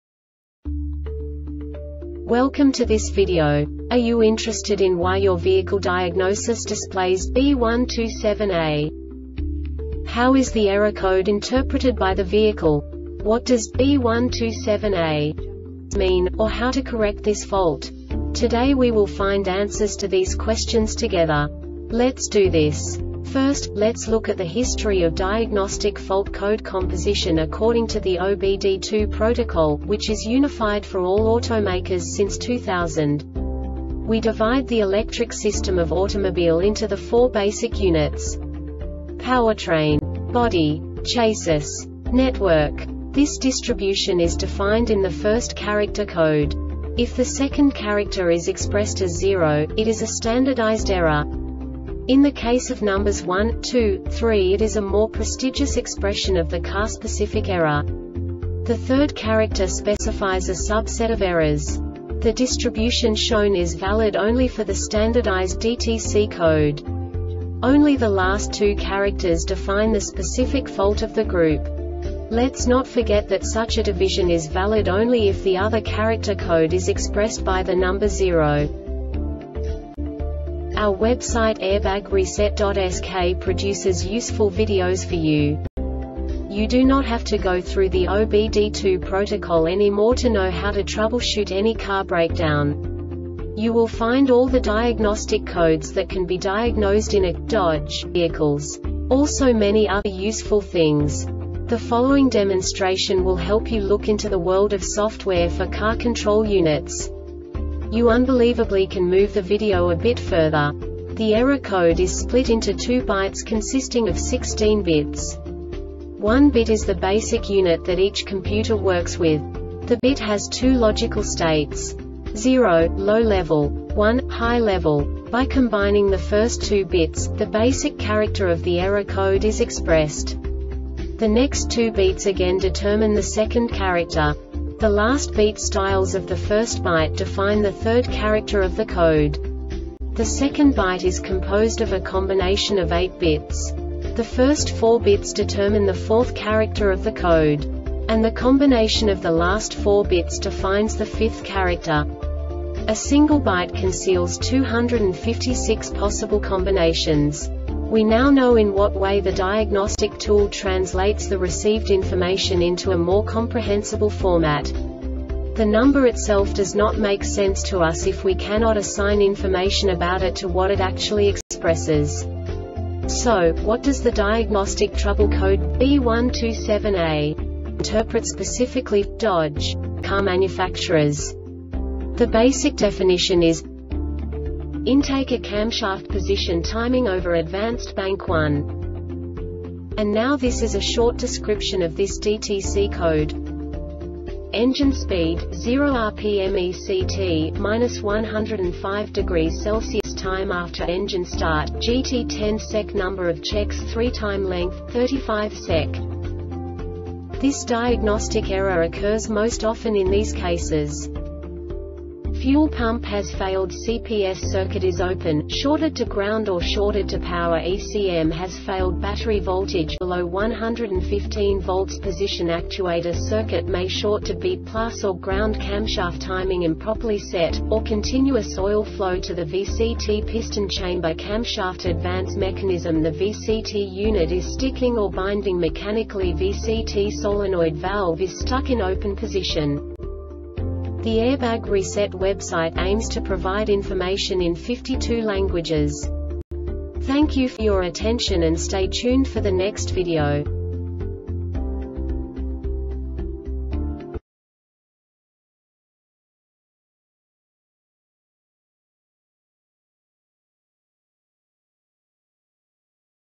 DTC Dodge B127A Short Explanation
Contents:
0:21 Basic DTC analysis according to OBD2 protocol standard.
1:48 Insight into programming
2:58 A diagnostically understandable description of the Diagnostic Trouble Code
3:05 Basic error definition

Make
Dodge
Code
B127A
Definition
Intake A Camshaft Position Timing Over-Advanced Bank 1
Description
Engine speed,  0 RPM  ECT,  -105° C  Time after engine start,   gt10 Sec   Number of checks 3  Time length,  35 Sec
Cause
 Fuel pump has failed CPS circuit is open,  shorted to ground or shorted to power ECM has failed Battery voltage below 115 volts Position actuator circuit may short to B+ or Ground Camshaft timing improperly set,  or continuous oil flow to the VCT piston chamber Camshaft advance mechanism the VCT unit is sticking or binding mechanicAlly VCT solenoid valve is stuck in open position